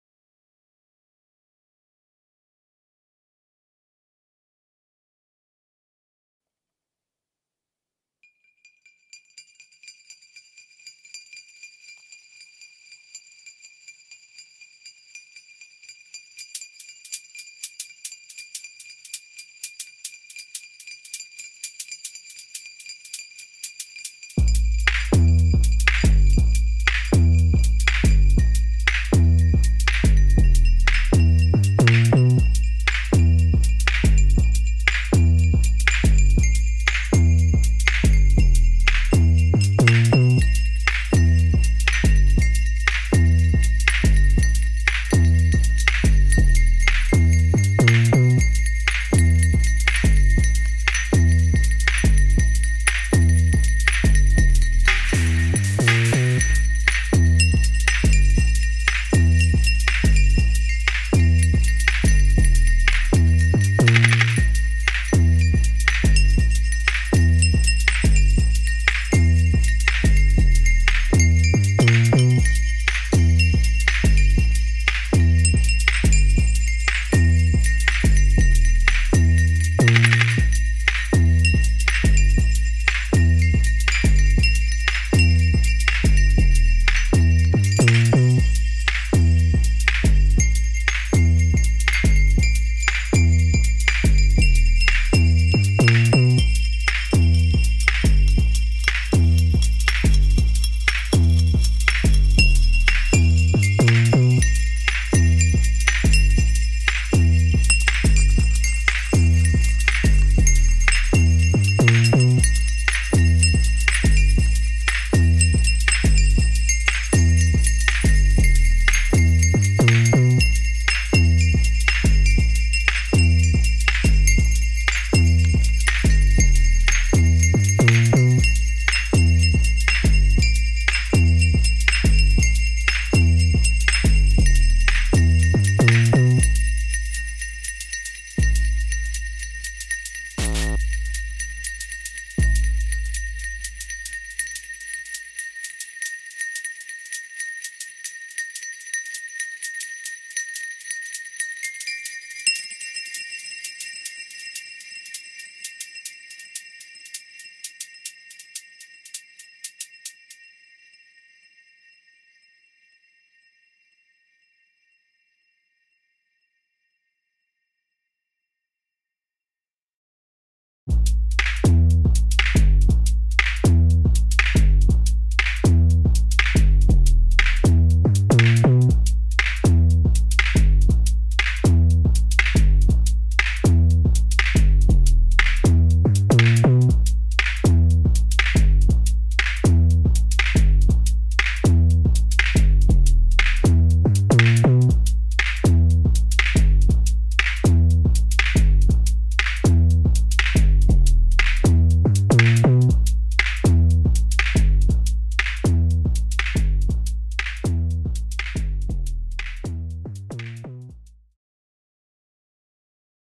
Jamuary2023 Jam 9th with a cheap toy xylophone, AUM with Rymdigare AUv3 plugin and TE PO-12 Rhythm
Jam 9th of Jamuary2023 is the one where my wife brings home an extra cheap toy xylophone which isn’t actually tuned and few of its pads don’t produce proper sound.

My wife says it is so cheap that we can sample it and give it away or, worst case, throw it out.

I figured it is a perfect opportunity for some acoustic experiments with one of my favourite iOS effect plugins - Rymdigare by humbletune

I think it turned out pretty weird, but i still hope you like it 😇

Charity of this jam is Rubikus e.V. - a team of volunteers helping Ukrainian refugees affected by the war travel to safety in Europe:

That’s not an easy task and you guys know what has to be done.

Gear:
🪗 a cheap malfunctioning toy xylophone
👾 Teenage Engineering PO-12 Rhythm
📷 iPhone 12 Pro running FiLMiC Pro app
📲 TC Helicon Blender and Roland GO:MIXER
🎙Zoom H5
🔈 AUM by Kymatica
🪄 Rymdigare and a bunch of free AUv3 plugins
🎞 LumaFusion